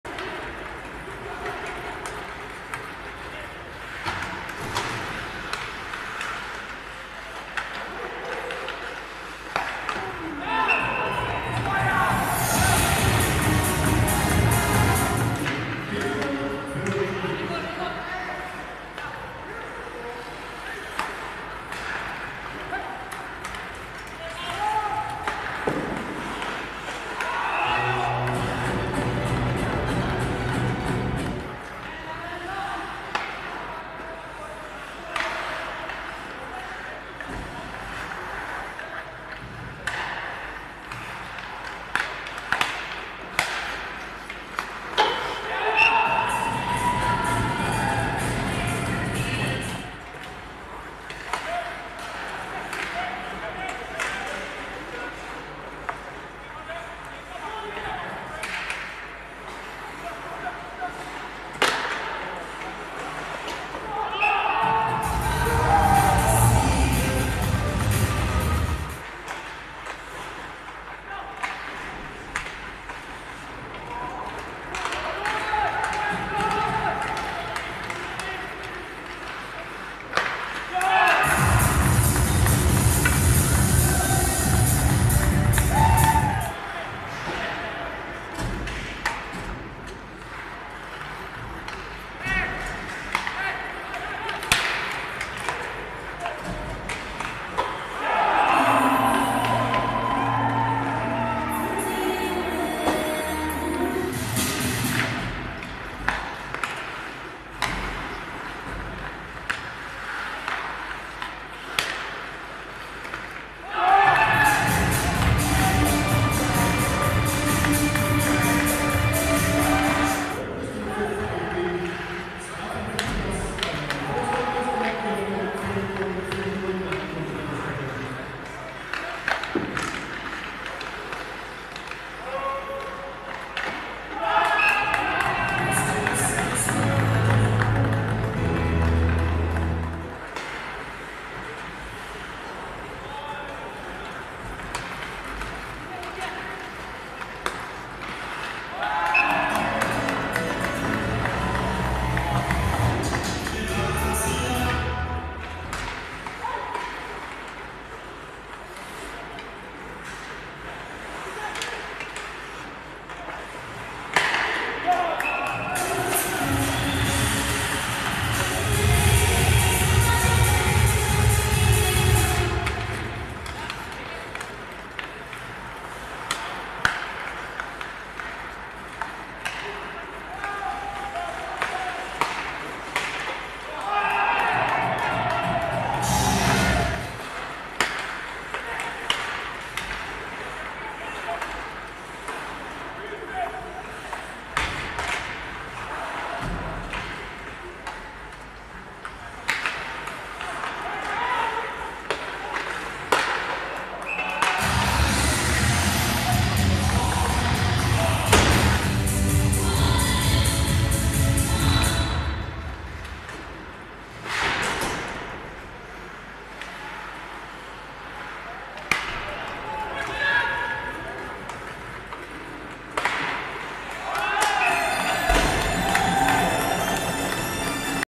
J20 Nationell Södra: Malmö Redhawks - HV71 3-10
Höjdpunkter från Malmö - HV71.
(2-2, 1-4, 0-4)
Skottstatistik: 26-39
Publik: 68
Matcherna sänds på SvenskHockey.TV